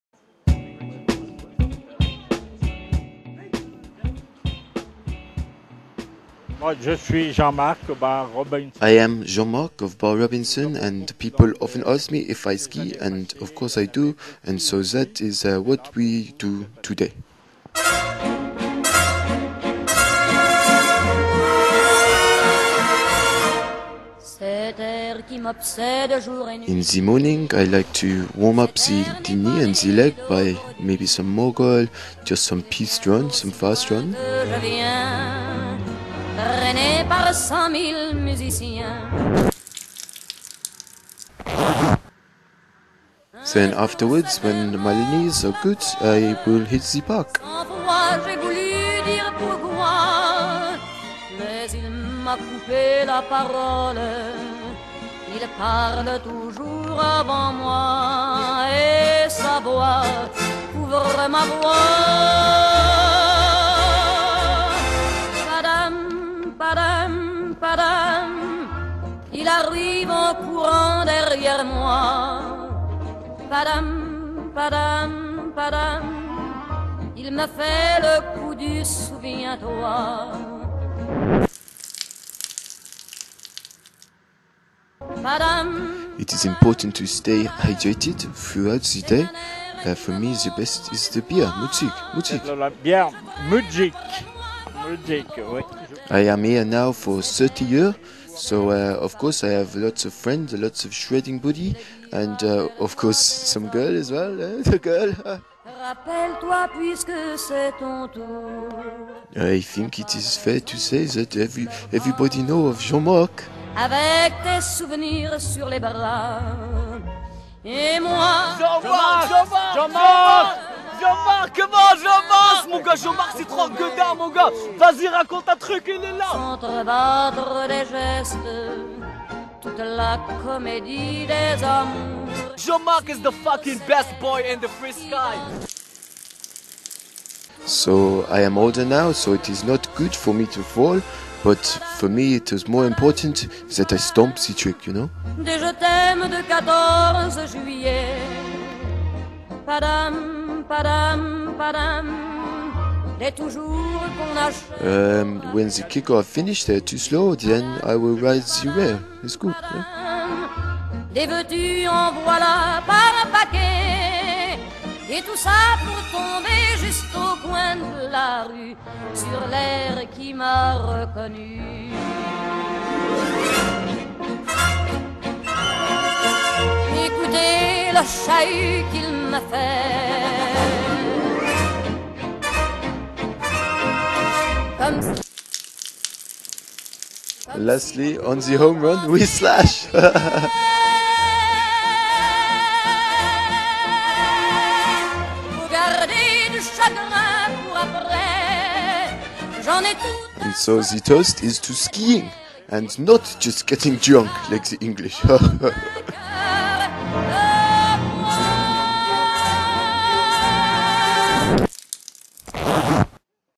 Cavern 24/7 - Jean-Marc Robinson
The legend of Morzine, Jean-Marc from Bar Robinson takes us for a day on the hill!